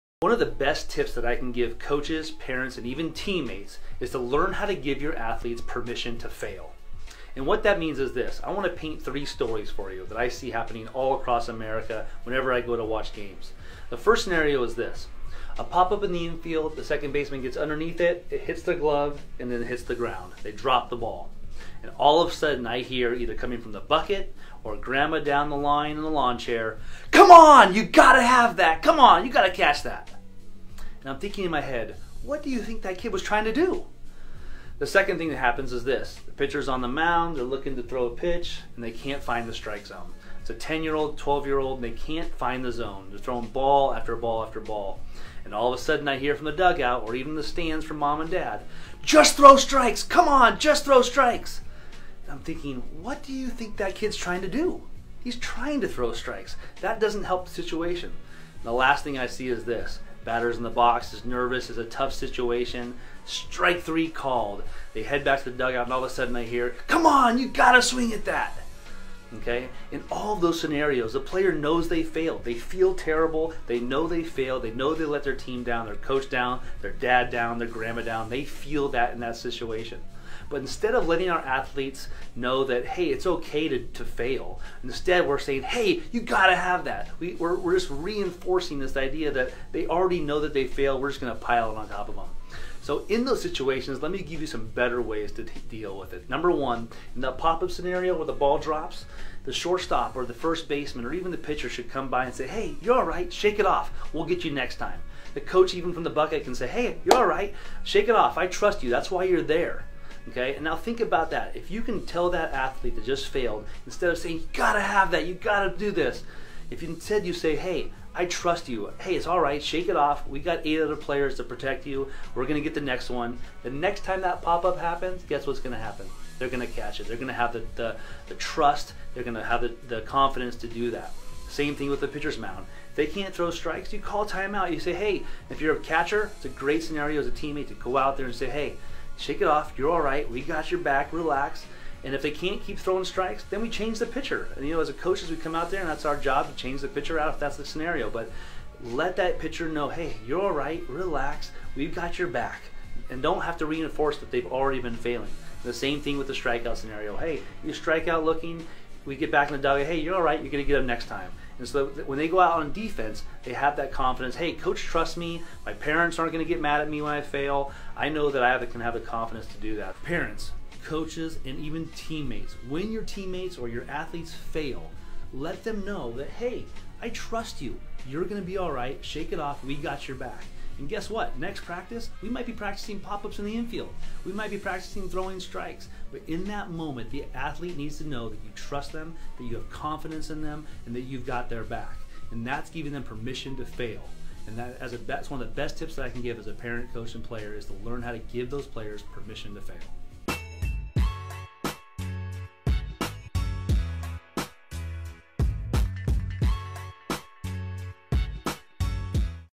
Permission To Fail
Parents/Coaches: Here is some of the best advice I can give you.

Follow on Facebook - Facebook.com/CoachLisle